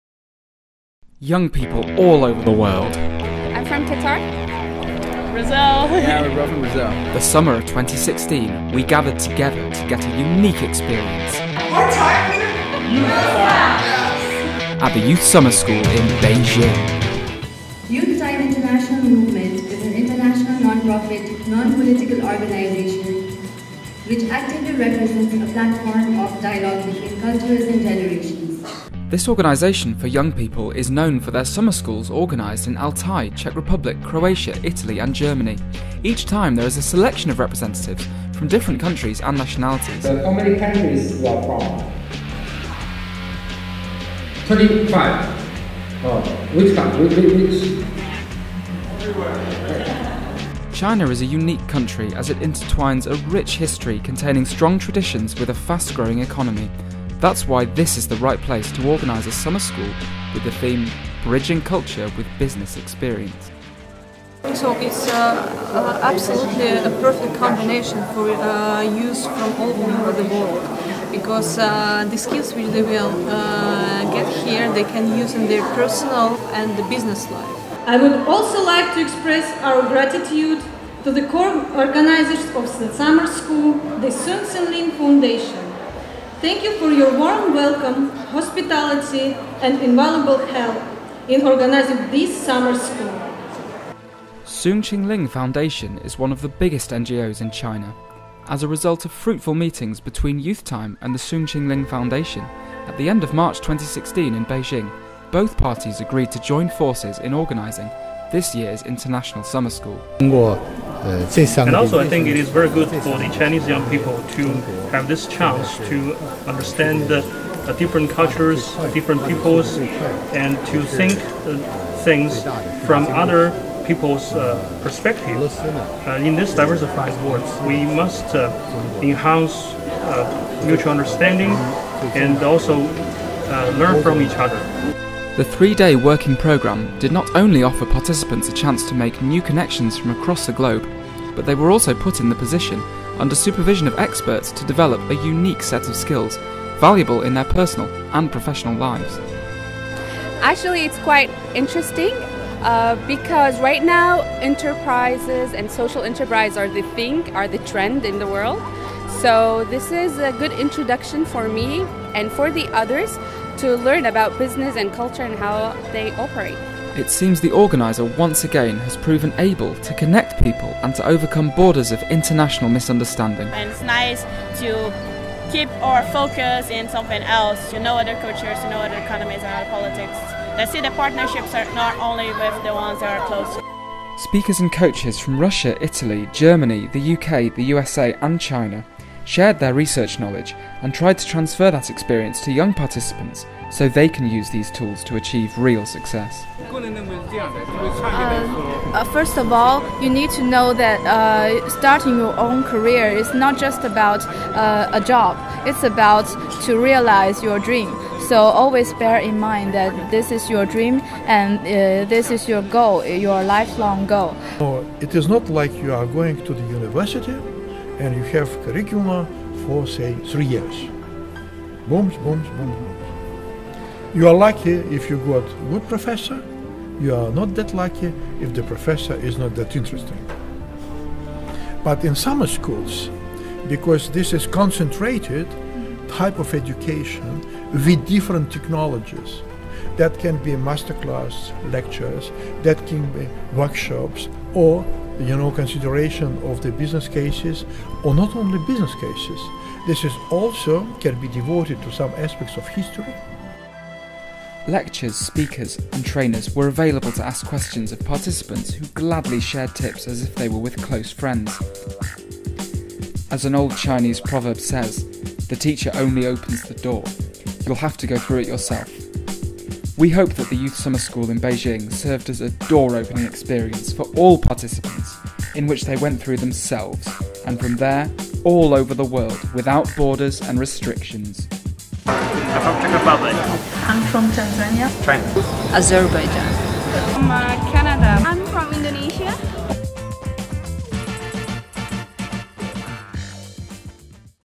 The International Youth Summer School in Beijing. Final report.
From the 15th till the 20th of August 2016, Youth Time organized its sixth consecutive Summer School. This year the event was realized in Beijing, in close cooperation with the reputable China Soong Ching Ling Foundation. It was Youth Time’s second Summer School organized outside of Europe and it has become a memorable one. Hosted in spectacular facilities, 30 international and 20 Chinese participants, coming in total from 25 different nations, were offered a rich cultural program and a diverse working schedule.

Creator/ Editor: Vasilisa Vershinina
Cameraman: Victor Patukov
Production manager: Yulia Gottstein